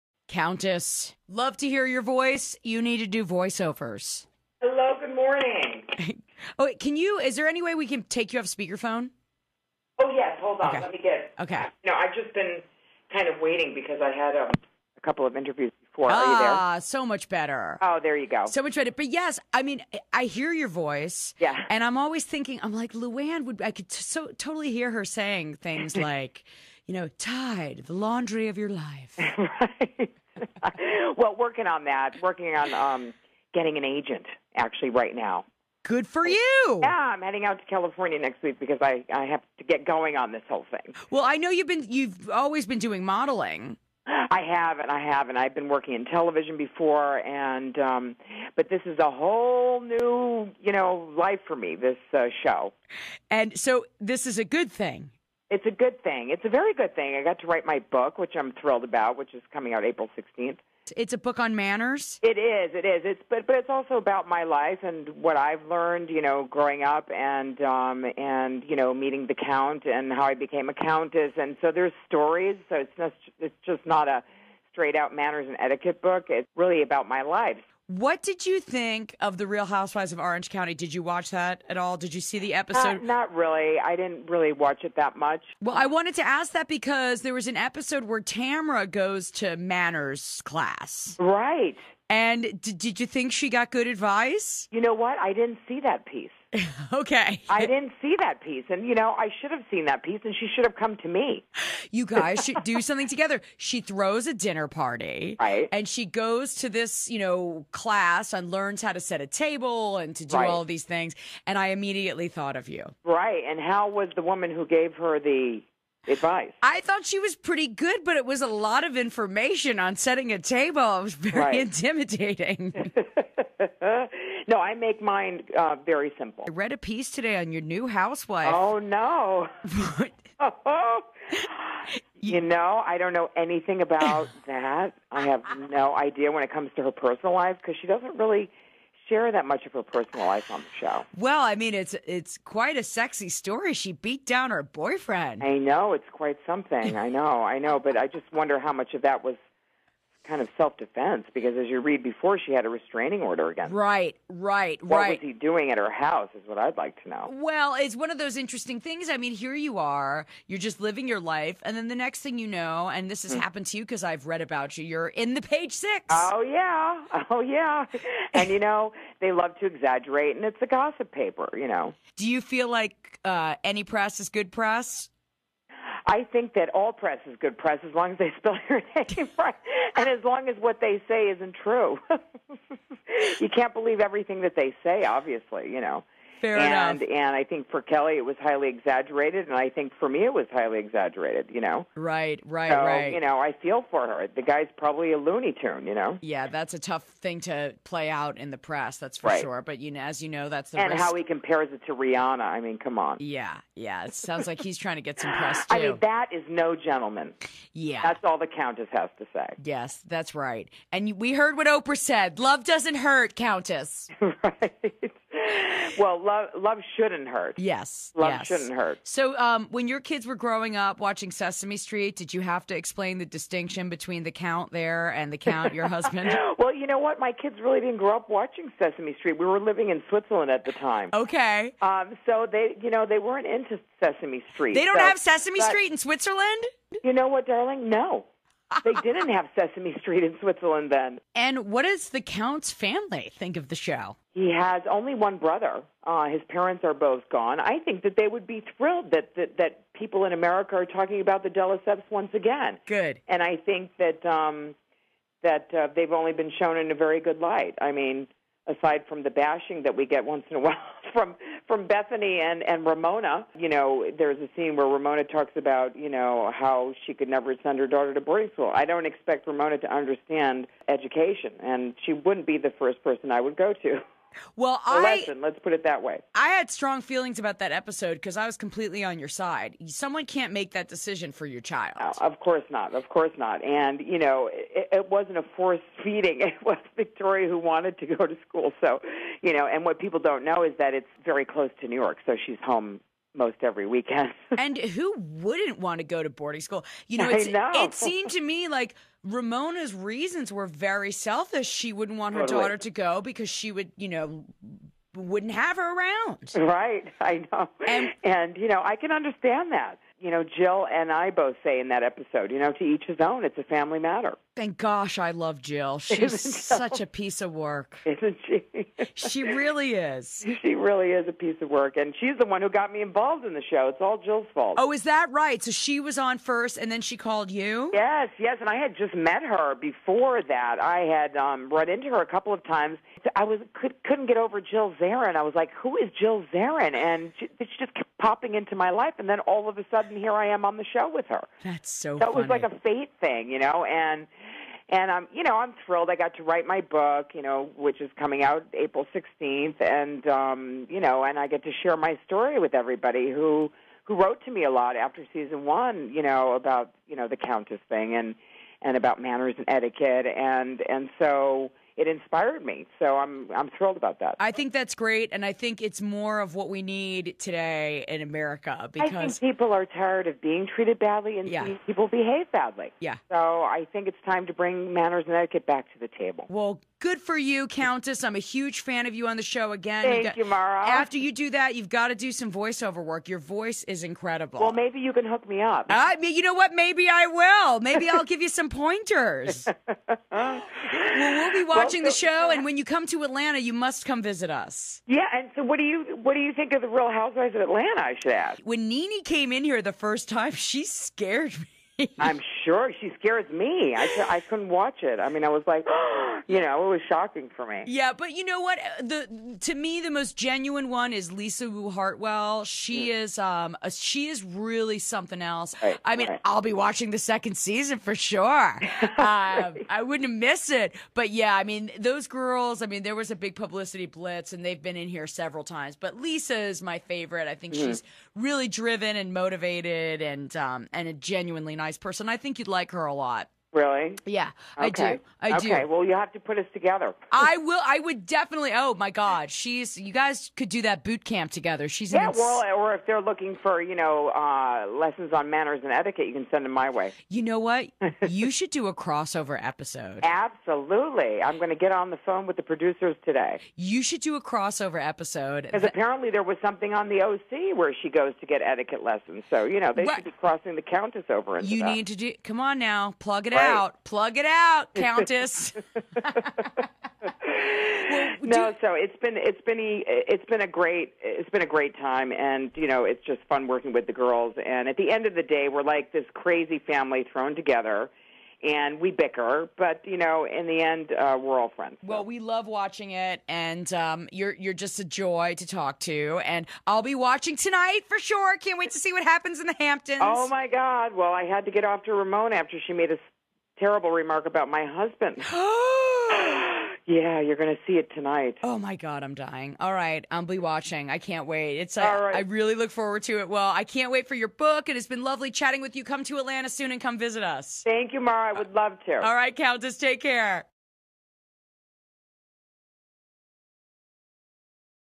Mara talks to The Countess from Real Housewives of NYC
Mara talks with Luann the Countess from the Real Housewives of New York.